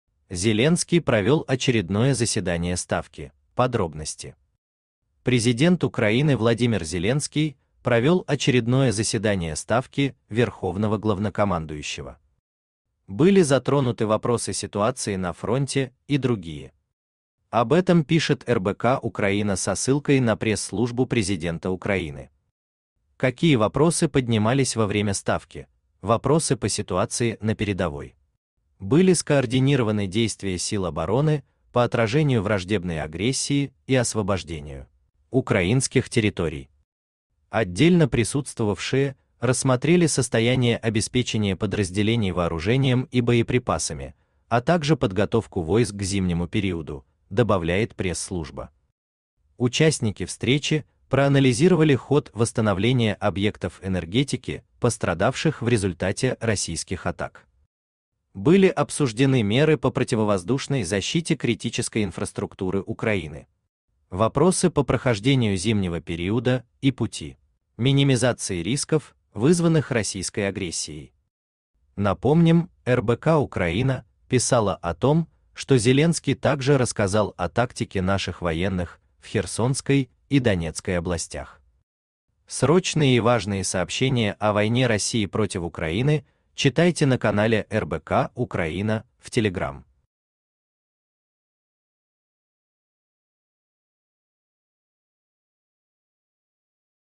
Зеленский провел очередное заседание Ставки: подробности.
РоссийскаяФедерация ВойнавУкраине ВладимирЗеленский Президент Украины Владимир Зеленский провел очередное заседание Ставки верховного главнокомандующего. Были затронуты вопросы ситуации на фронте и другие. Об этом пишет РБК-Украина со ссылкой на пресс-службу президента Украины. Какие вопросы поднимались во время Ставки:вопросы по ситуации на передовой. Были скоординированы действия сил обороны по отражению враждебной агрессии и освобождению украинских территорий;. "Отдельно присутствовавшие рассмотрели состояние обеспечения подразделений вооружением